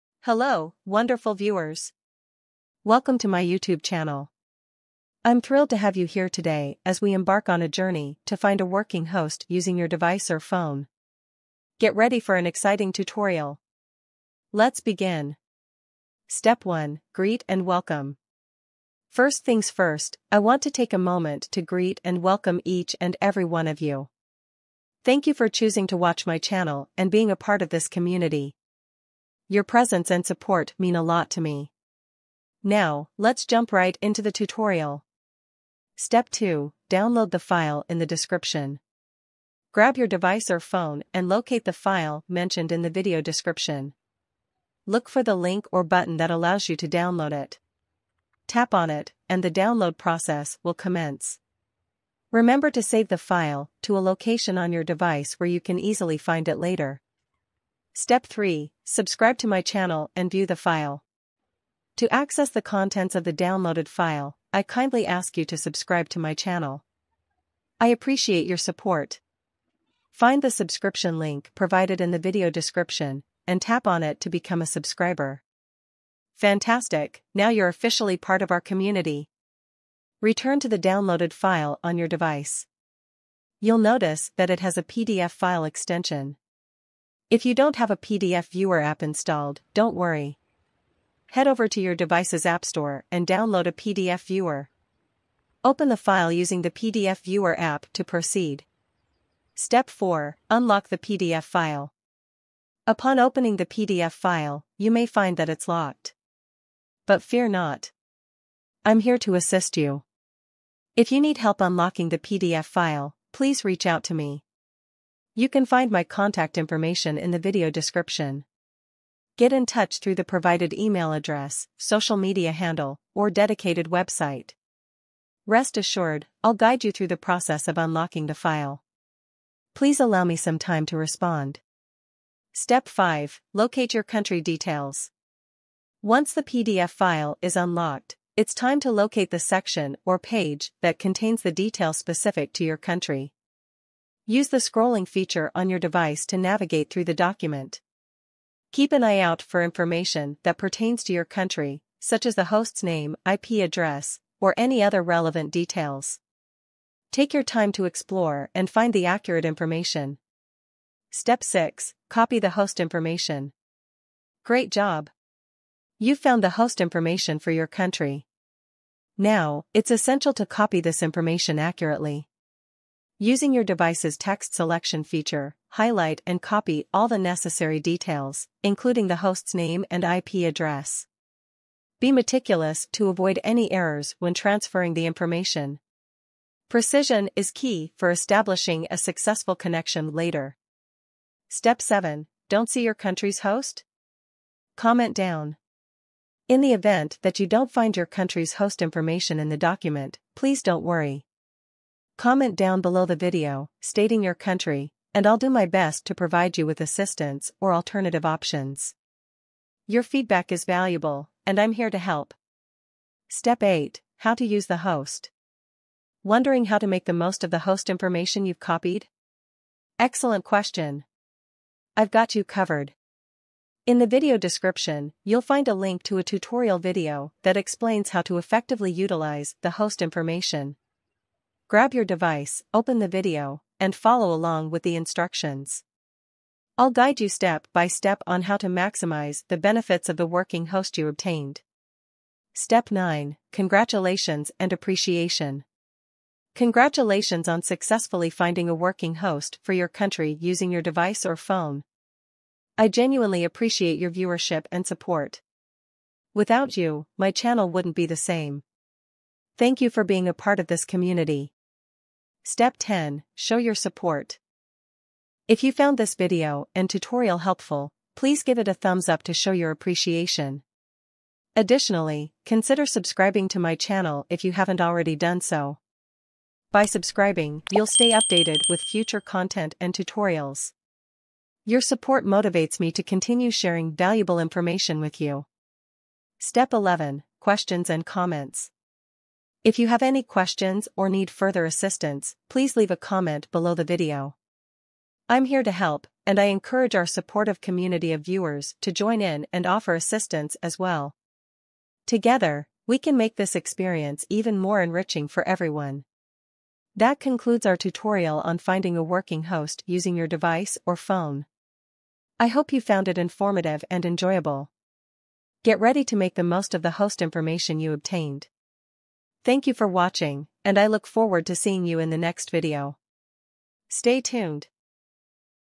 How to get a working host new method/bug sni give away
"Hello, wonderful viewers! I'm thrilled to have you here today as we embark on a journey to find a working host using your device or phone. Get ready for an exciting tutorial! Let's begin.

All websites links are put in a PDF file,